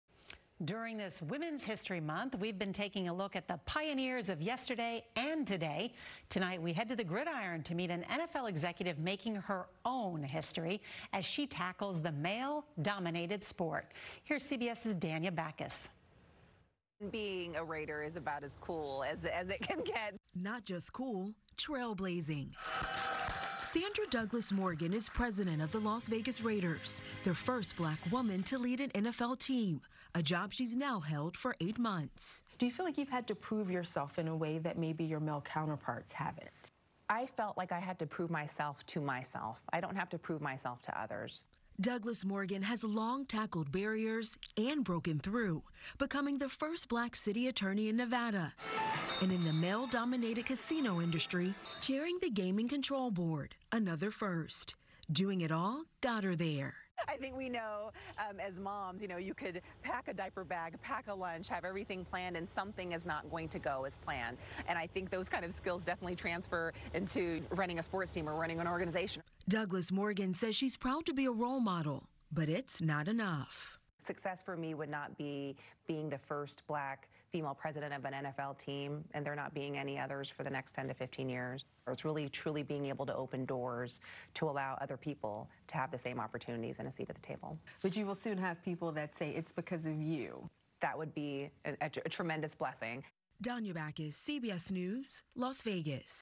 Sandra Douglass Morgan breaks barriers as first Black woman to be president of NFL team
Sandra Douglass Morgan last year became the first Black woman to be named president of an NFL team. As the head of the Las Vegas Raiders, Morgan continues to tackle challenges. Danya Bacchus takes a look at her story for Women's History Month.